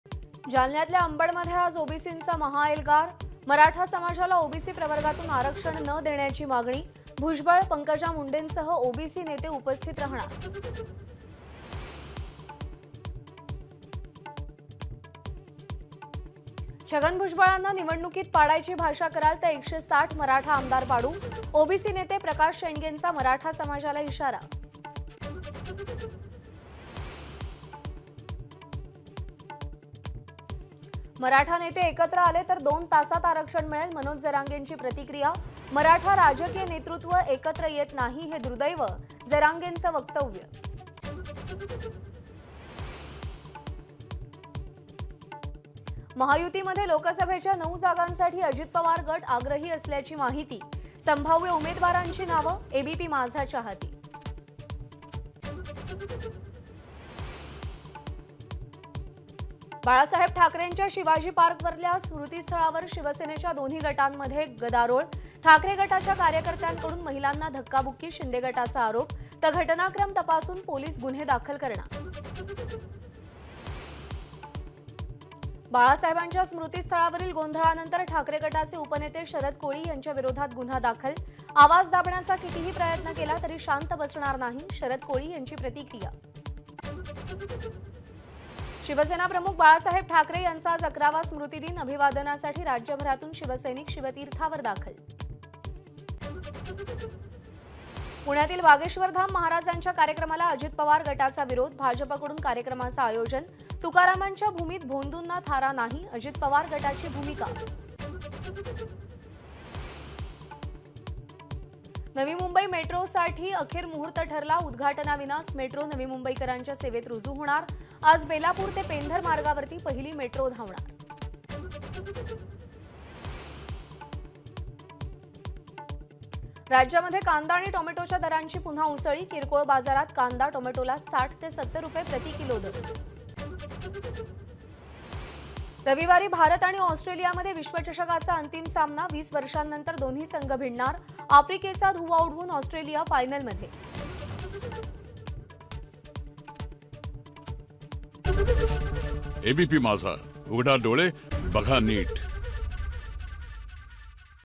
ABP Majha Marathi News Headlines 10 AM TOP Headlines 10 AM 17 November 2023
ABP Majha LIVE | Balasaheb Thackeray Death Anniversary | Thackeray vs Shinde | Manoj Jarange Patil